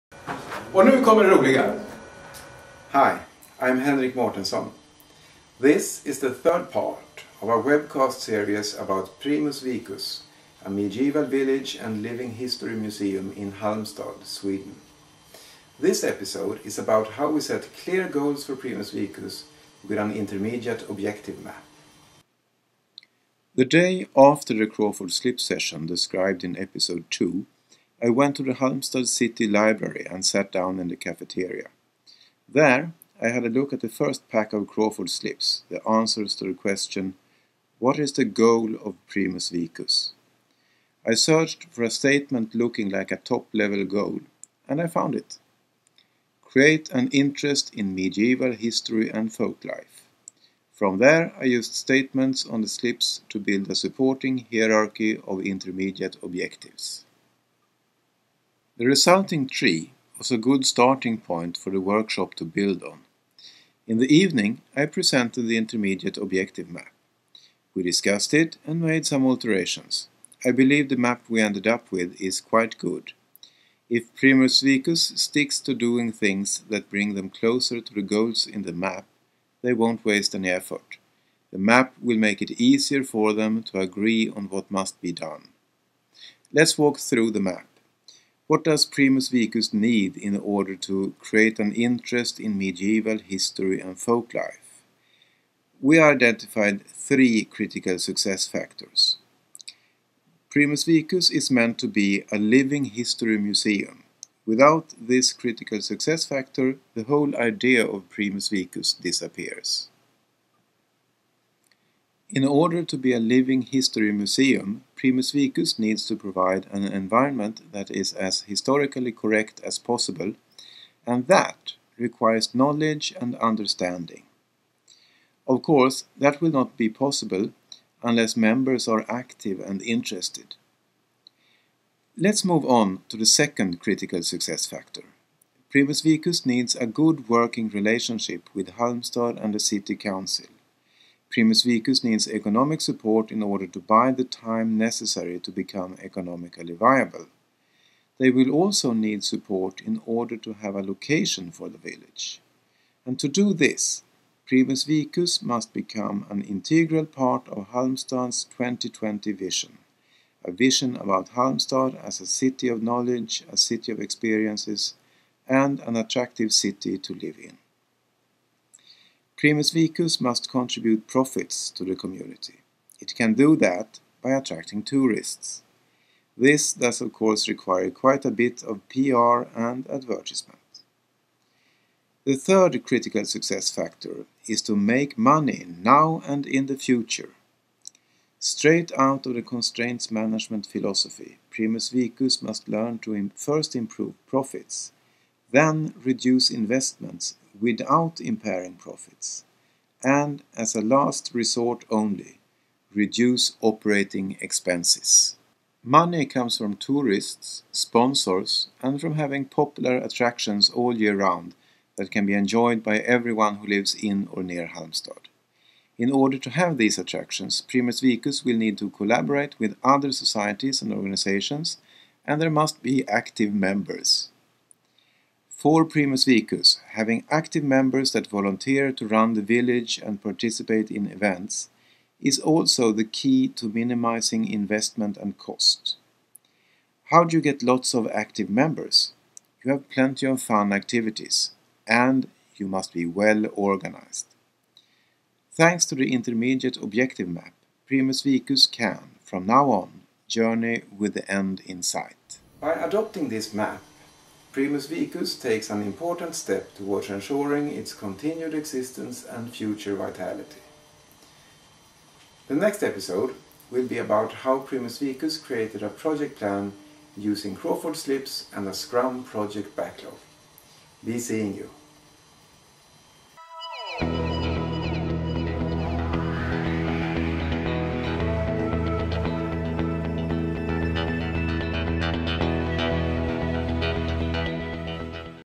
The Primus Vicus Project Part Three: The IO Map
This is part three of a series on a Crawford Slip brainstorming workshop I held at Primus Vicus, a medieval village and Living History museum in Halmstad, Sweden.

This part shows the Intermediate Objective Map we built from information gathered using Crawford Slip brainstorming.

My thanks to Primus Vicus for allowing me to videotape the project and use all material in webcasts and in my writings.